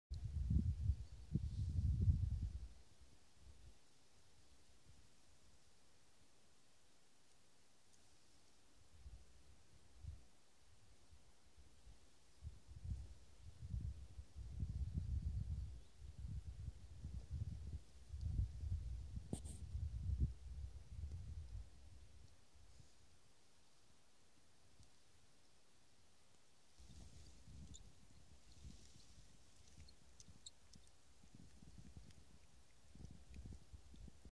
Färdig för håven!
Fin röding tagen på spinn!
I trakterna av Idre
C&R
Spinnare Tondo Bolt.